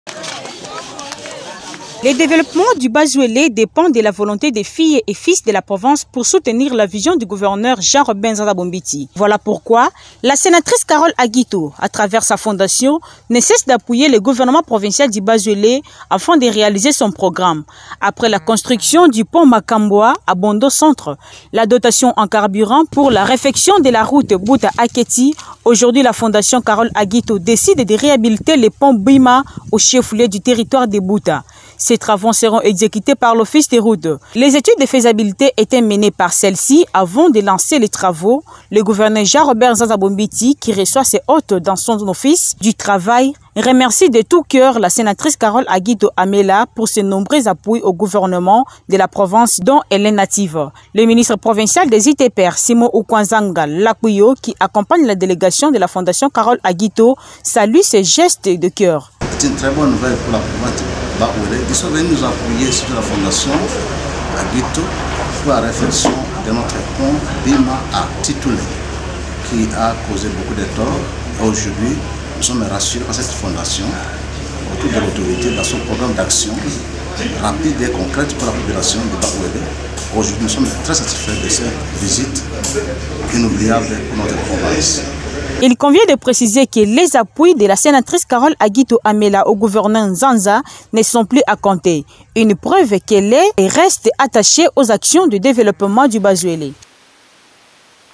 bas-uelenews_Contrat de réhabilitaion du pont Bima dans le territoire de Buta.
La fondation Carole Agito Amela, FCAA, va financer les travaux de réhabilitation du pont BIMA situé à 132 Kilomètres de Buta. Les travaux seront exécutés par l’Office des routes. Le gouverneur de province, le professeur Jean-Robert NZANZA Bombiti a été informé lors de l’audience qu’il a accordé aux membres de cette fondation.

Il y a donc lieu de dire que le développement du Bas-Uélé dépend de la volonté des filles et fils de la province pour soutenir la vision de gouverneur NZANZA Bombiti Jean-Robert. Raison pour laquelle la sénatrice Carole Agito, à travers sa fondation, ne cesse d’appuyer le gouvernement provincial du Bas-Uélé afin de réaliser son programme. Après la construction du pont Makamboa à Bondo centre, la dotation en carburant pour la réhabilitation de la route Buta – Aketi , aujourd’hui la fondation Carole Agito decide de réhabiliter le pont Bima au chef lieu du territoire de Buta. Ces travaux seront exécutés par l’Office des routes. Les études de faisabilité étaient déjà menées par cette fondation avant de lancer les travaux.